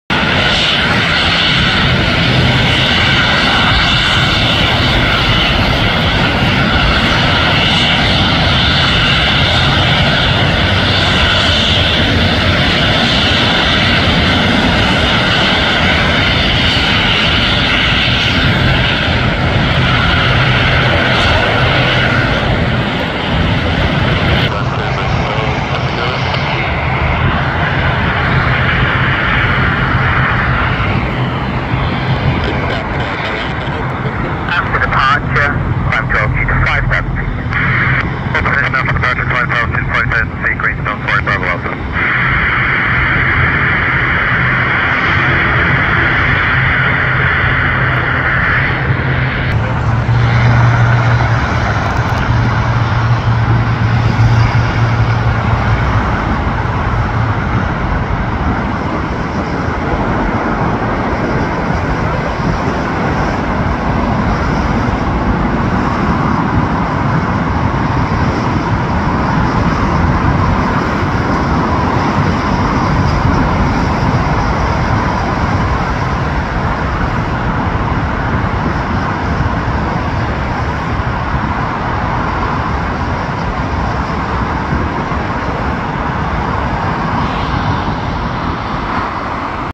Aer Lingus Regional AT76 Leaving Belfast 19/4/23
This one took off at runway 04, and filmed from the bridge.